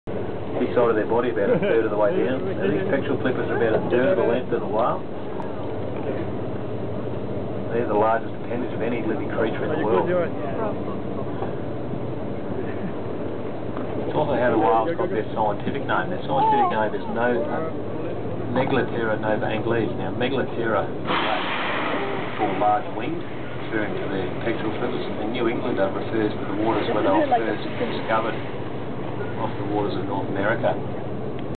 Whale Watching in Brisbane
I went whale watching at Moreton Bay Brisane on 10 August 2009.